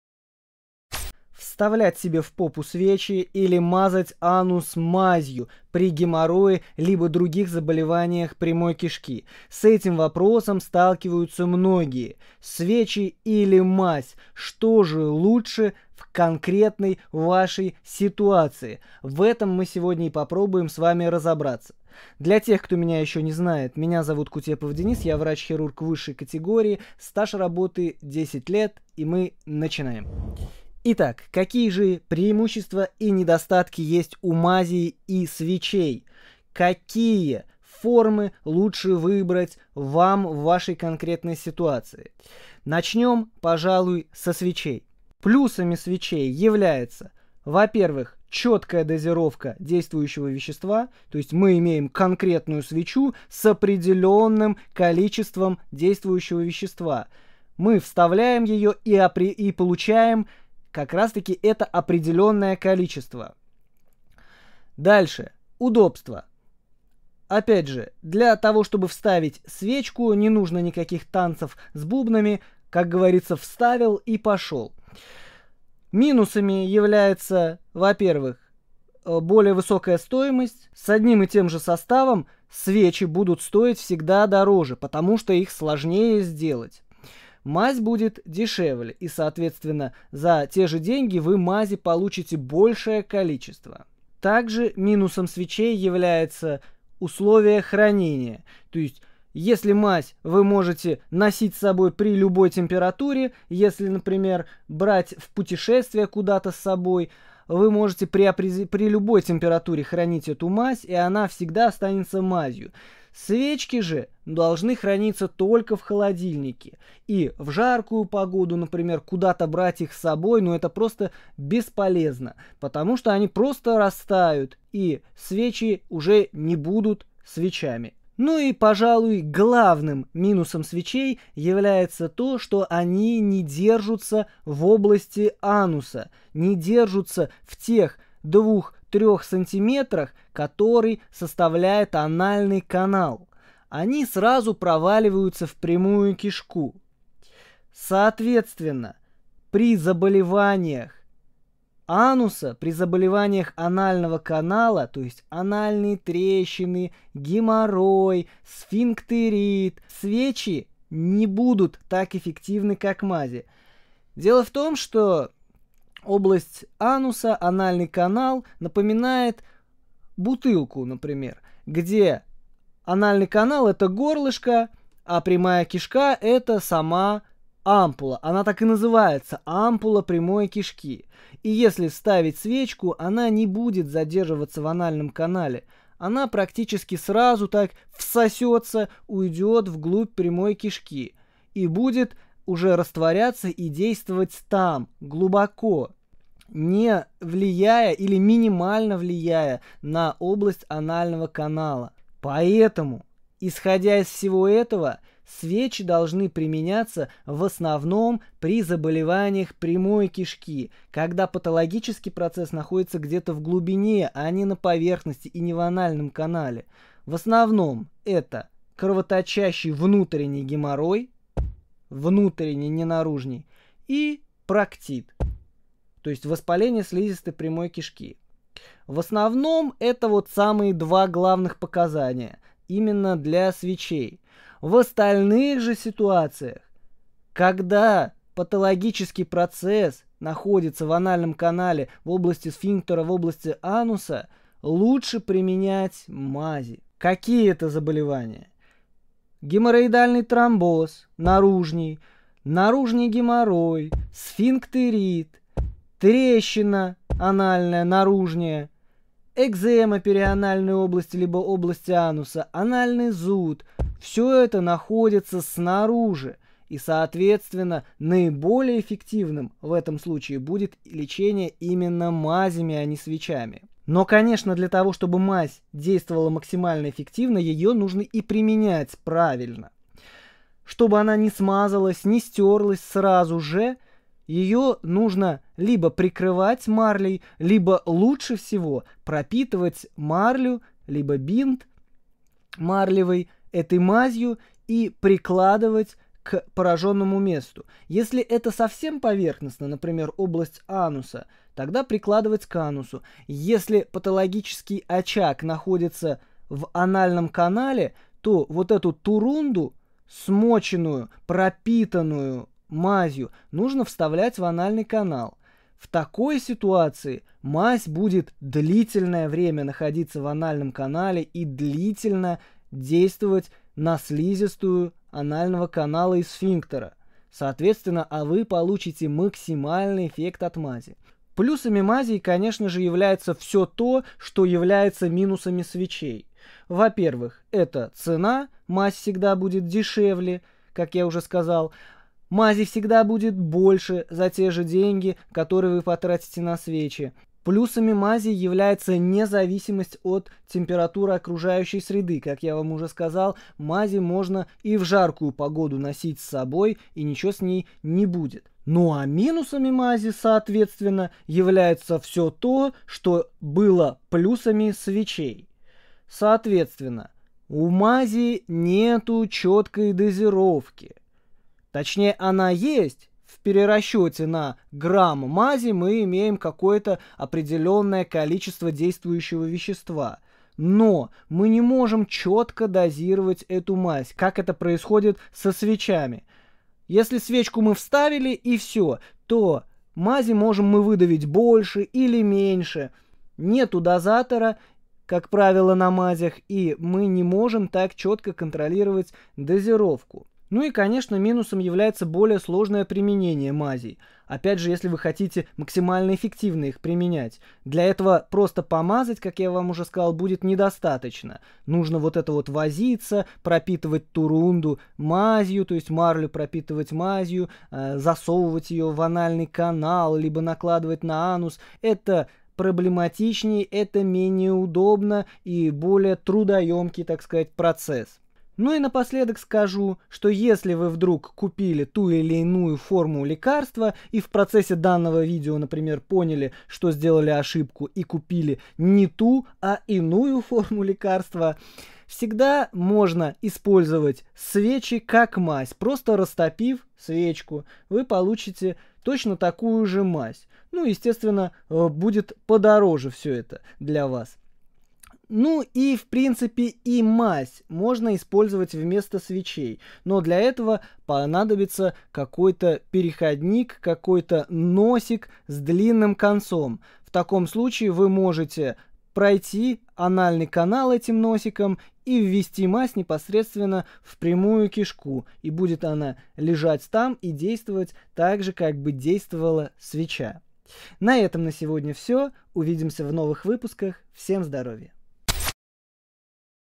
Мазь от геморроя | Свечи от геморроя | Лечение геморроя
Врач хирург высшей категории с большим опытом в проктологии рассказал всё про свечи от геморроя и мазь от геморроя! ЧТО ЛУЧШЕ?Что же выбрать ВАМ, в конкретно вашей ситуации? Если Вас интересует лечение геморроя, какие свечи или мазь при геморрое выбрать - это видео для Вас!  Если у Вас геморрой, сфинктерит, проктит, анальная трещина, анальный зуд, геморроидальный тромбоз, кровотечение из прямой кишки, какие либо другие заболевания  прямой кишки или просто боль в заднем проходе - обязательно посмотрите это видео! Не каждый проктолог расскажет об этом!